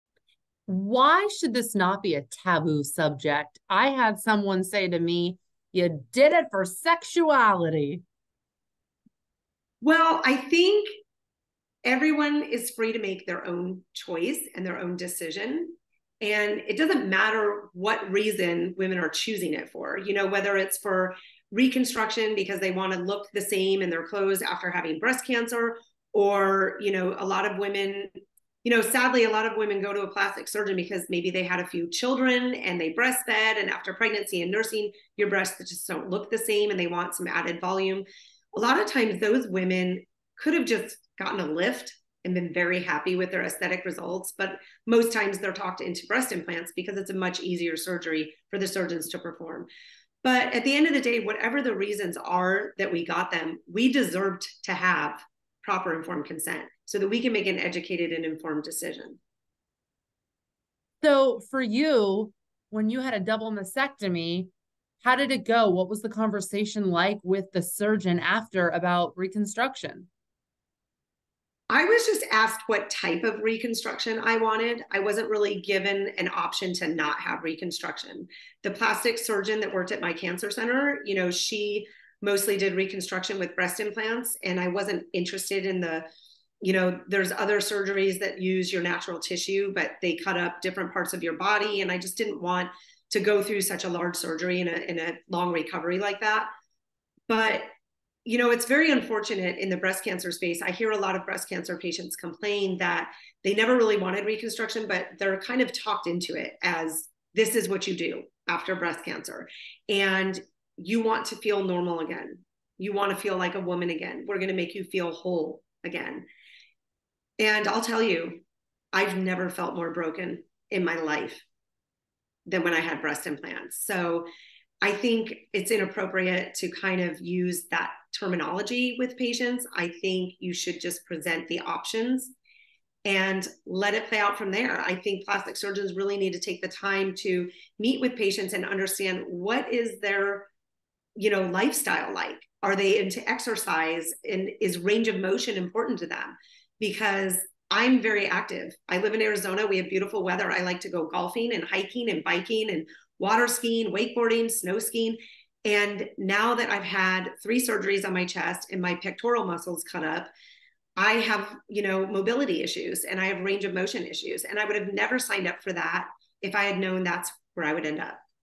Breast implants after cancer
Robyn Towt is a three-time cancer survivor who says, she was harmed by a medical device. After a double mastectomy, she had breast implant surgery. Four months later, Robyn explanted.